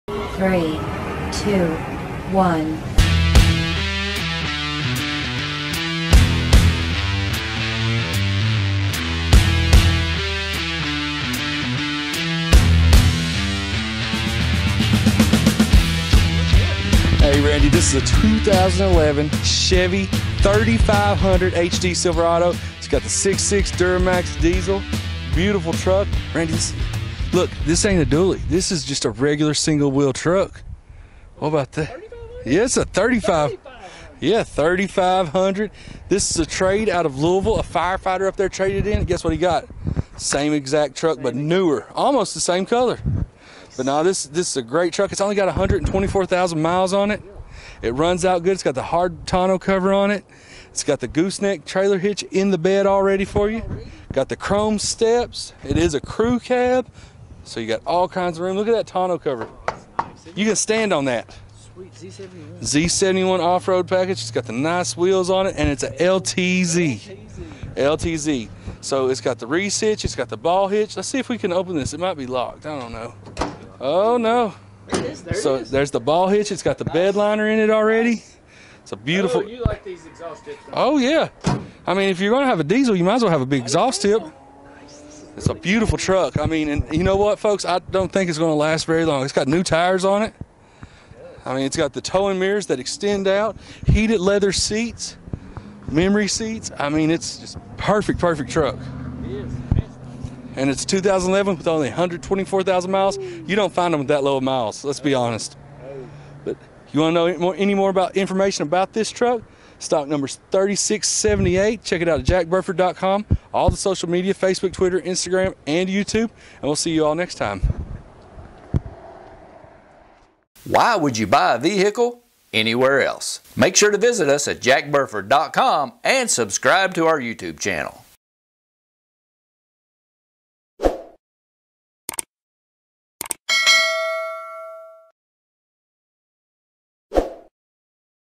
USED 2011 Chevrolet Silverado 3500HD LTZ
Pre Owned 2011 Chevrolet Silverado 3500HD Crew Cab Standard Box 4 Wheel Drive Single Rear Wheel LTZ Check it out